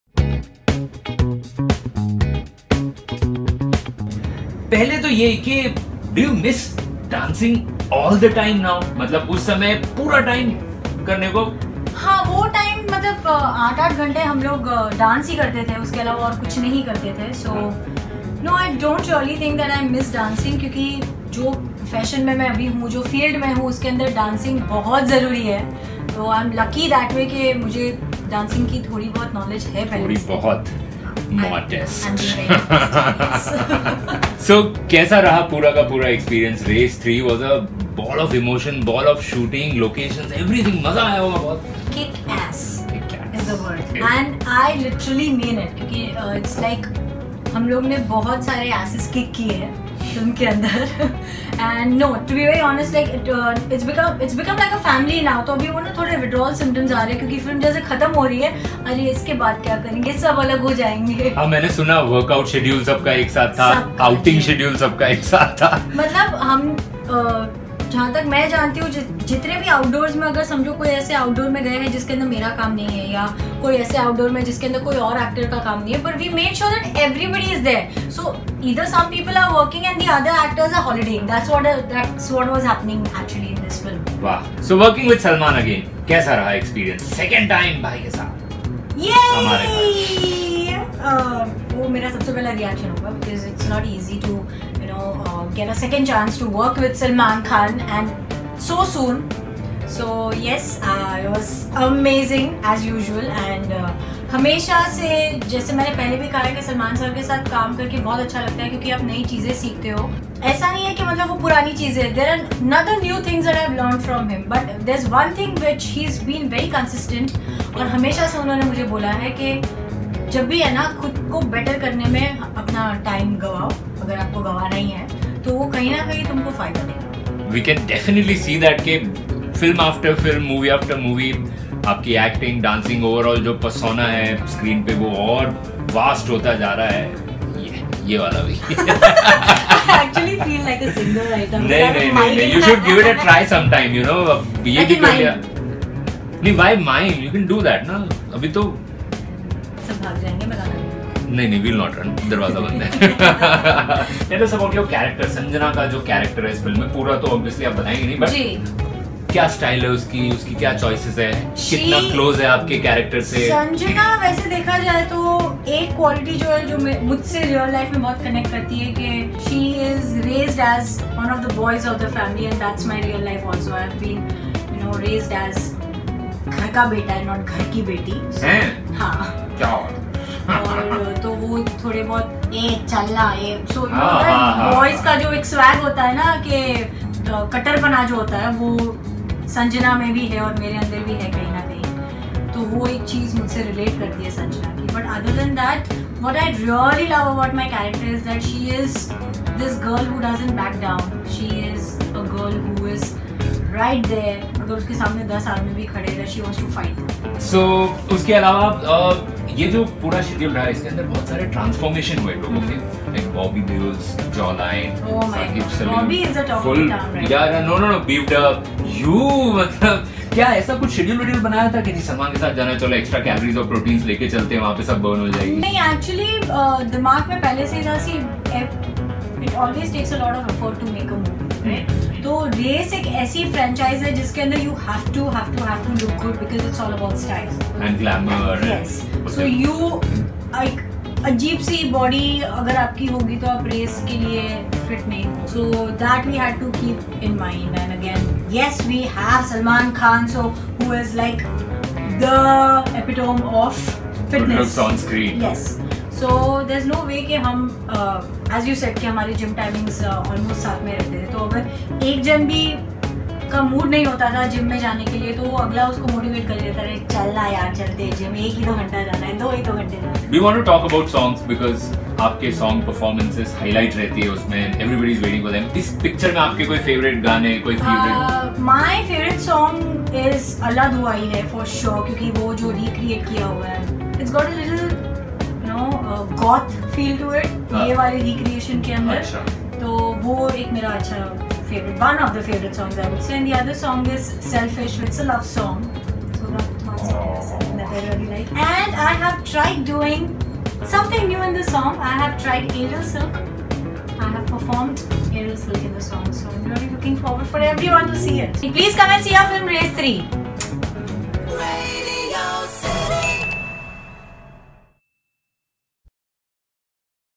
EXCLUSIVE- Race 3 Star Daisy Shah's Interesting Revelation About Salman Khan
Daisy Shah shares her experience of working with Salman Khan for the second time!